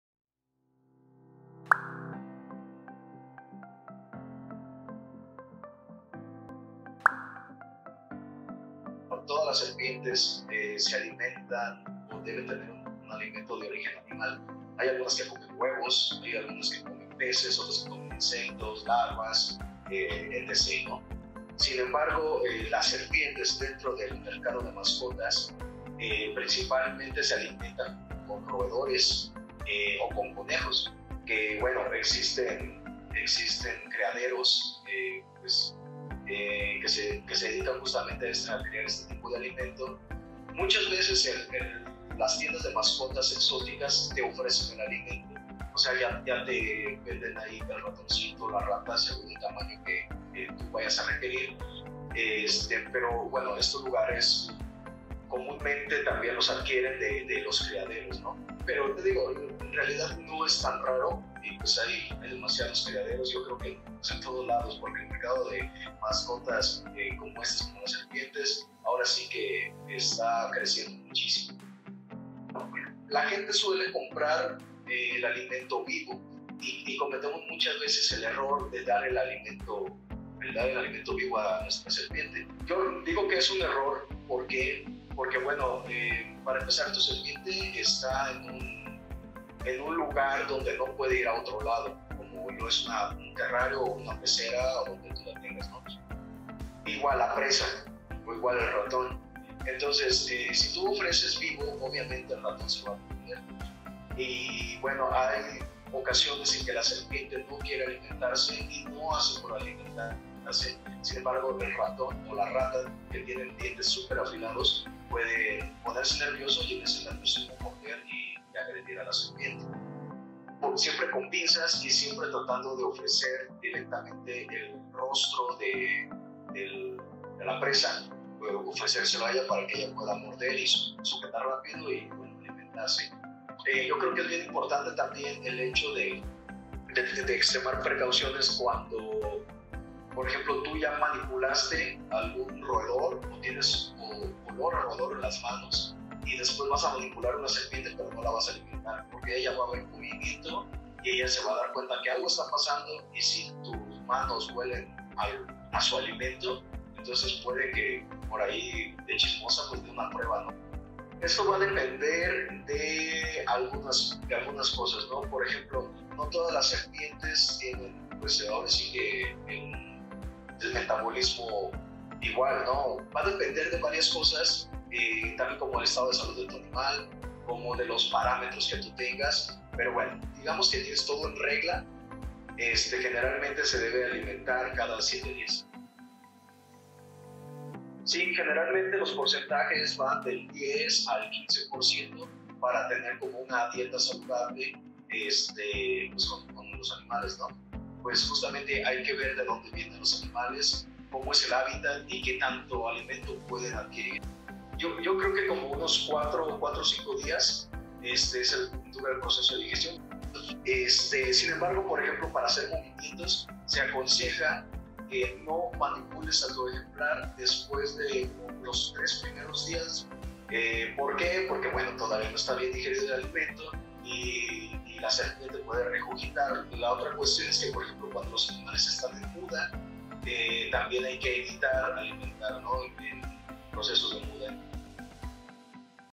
Alimentación en serpientes 🐍| ¿Qué alimentos y cada cuanto puede comer mi SERPIENTE?
En general, las serpientes son conocidas por su extraordinaria velocidad, manera de cazar y tragar de un solo bocado a sus presas cuando se encuentran en su hábitat natural 🐍😲 pero cuando son criadas en cautiverio el dueño se convierte en el responsable de proporcionarle su alimento.

¡Quédate a ver el siguiente video! En donde el Biólogo Juan Manuel González, experto en estos reptiles nos comparte algunos consejos de comida que le puedas brindar a tu serpiente 🐍👀👇